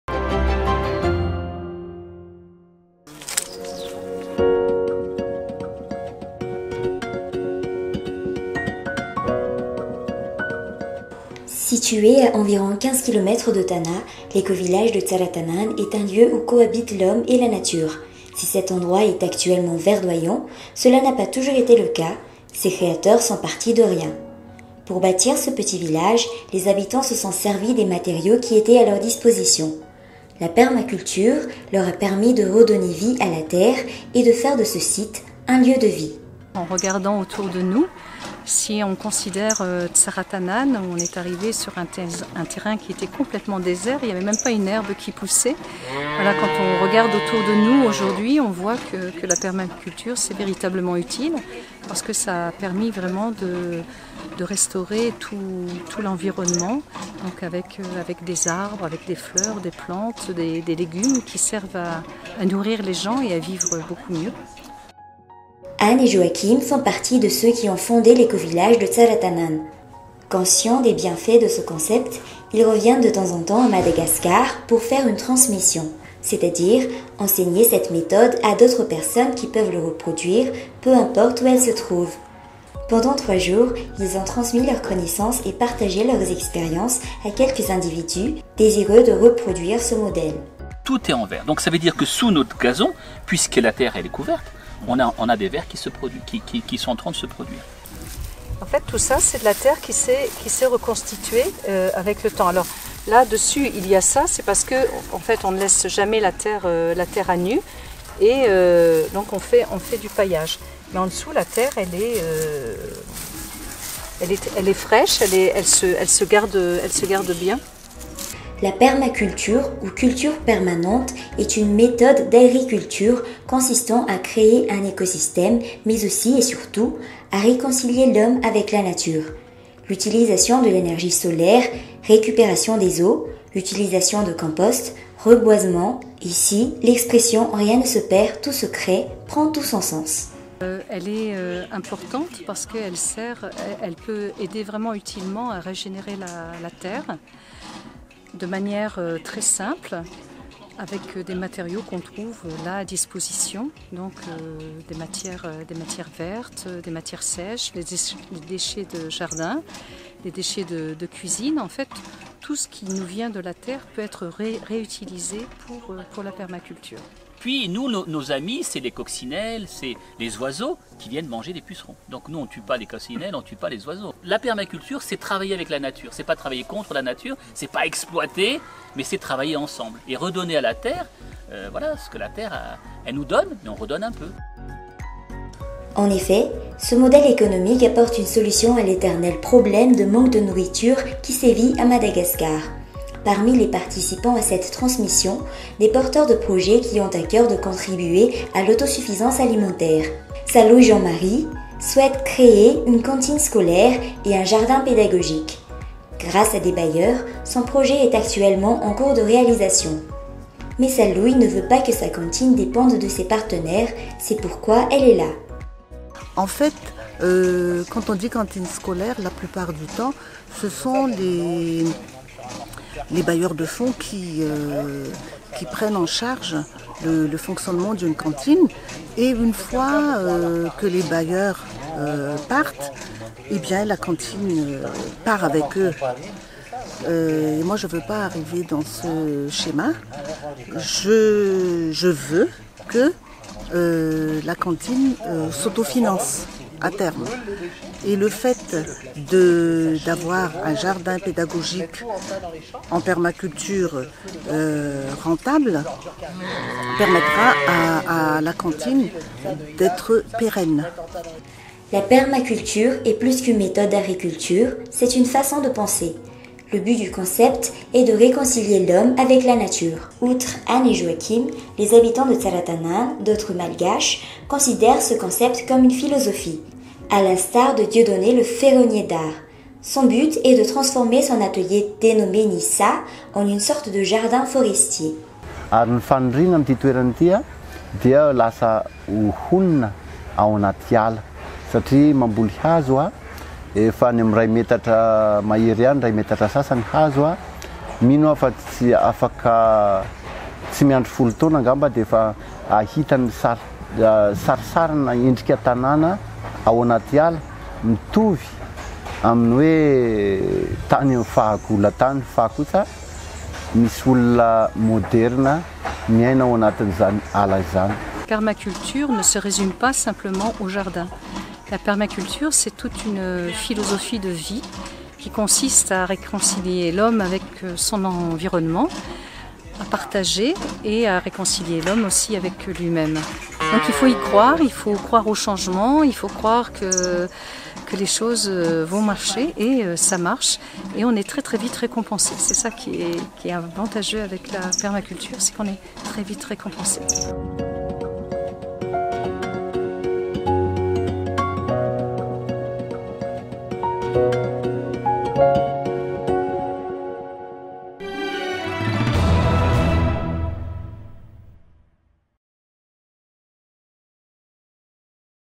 LA PERMACULTURE : PLUS QU'UNE AGRICULTURE, UNE FAÇON DE PENSER
LA PERMACULTURE - Situé à environ 15 km de Tanà, l’écovillage de Tsaratanàna est un lieu où cohabitent l’homme et la nature. Si cet endroit est actuellement verdoyant, cela n’a pas toujours été le cas ; ses créateurs sont partis de rien. Pour bâtir ce petit village, les habitants se sont servis des matériaux qui étaient à leur disposition. La permaculture leur a permis de redonner vie à la terre et, de faire de ce site « un lieu de vie ».